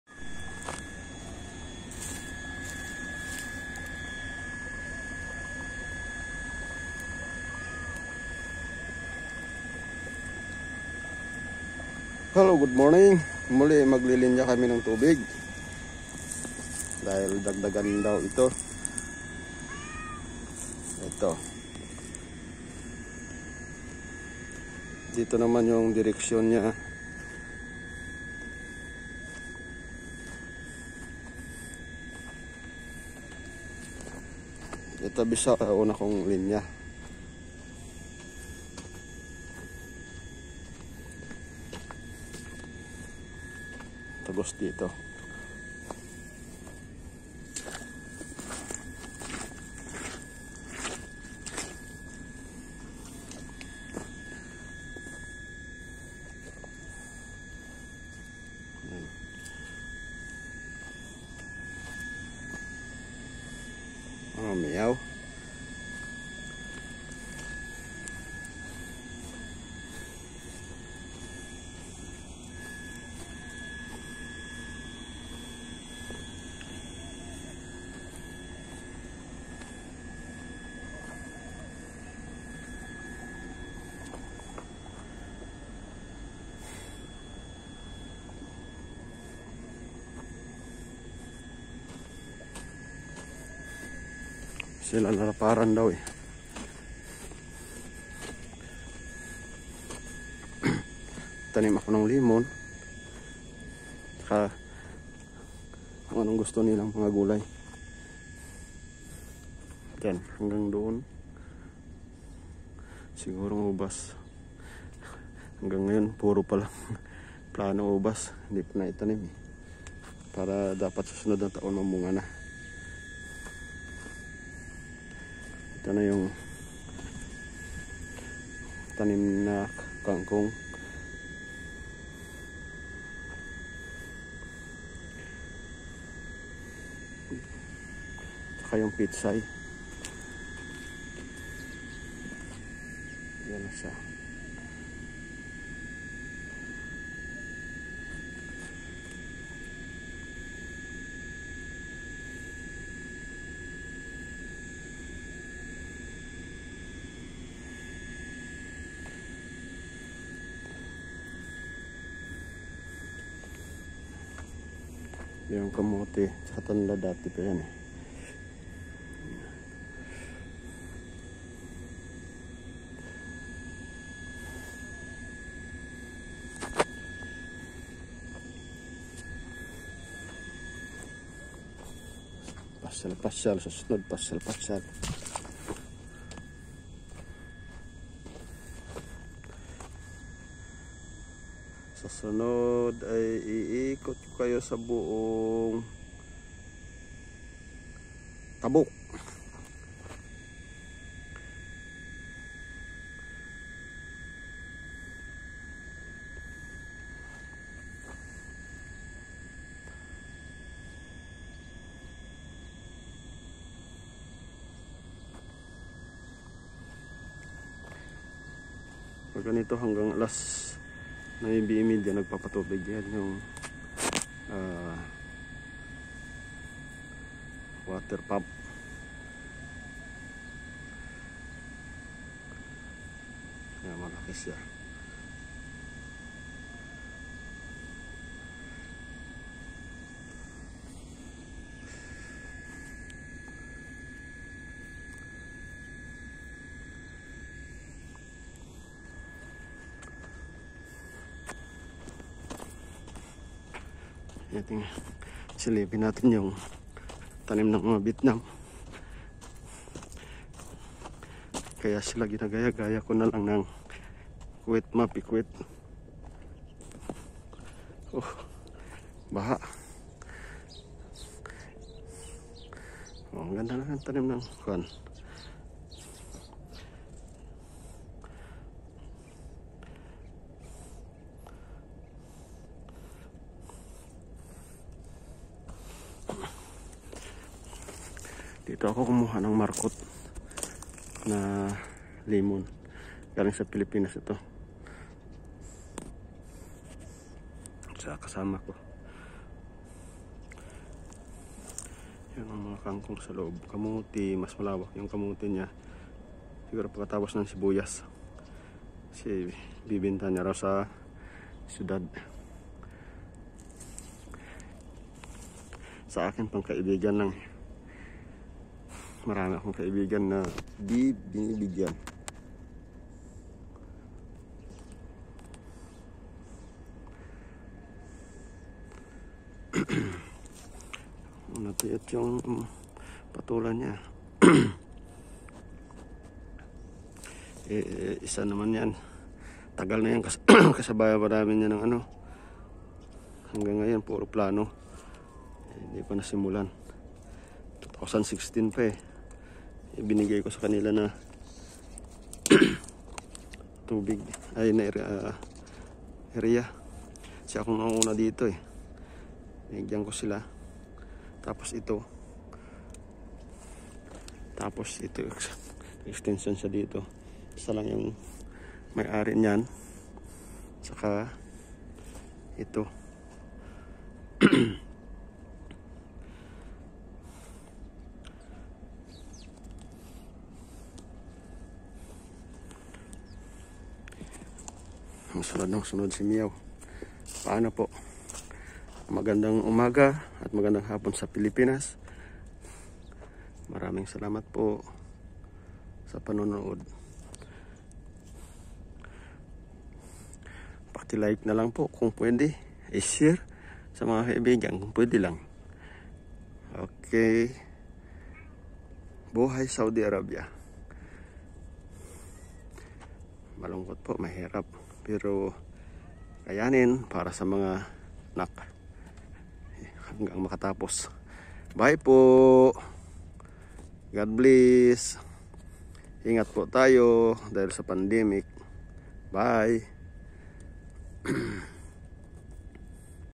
PANIBAGONG LINYA NG TUBIG
NAG LINYA KAMI ULIT PARA DAGDAGAN O HABAAN ANG LINYA PARA DI KALADKARIN NG TUBIG ANG LUPA